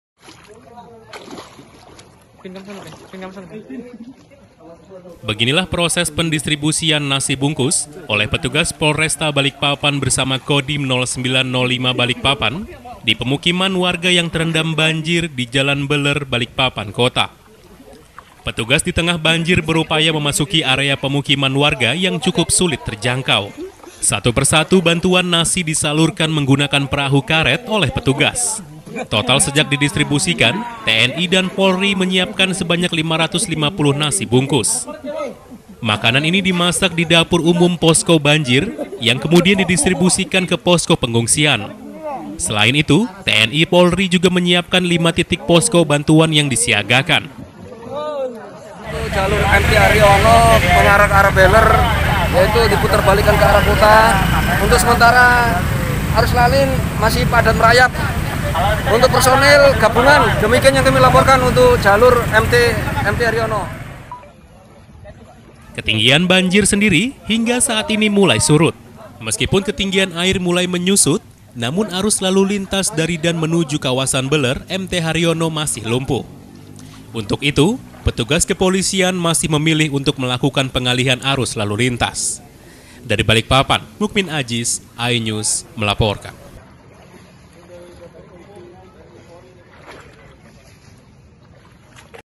Distribusi Bantuan Dengan Perahu Karet
Bantuan kepada puluhan warga korban banjir yang masih bertahan di dalam rumah mulai disalurkan. Sebanyak 500 nasi bungkus, didistibusikan langsung oleh gabungan TNI dan Polri menggunakan perahu karet.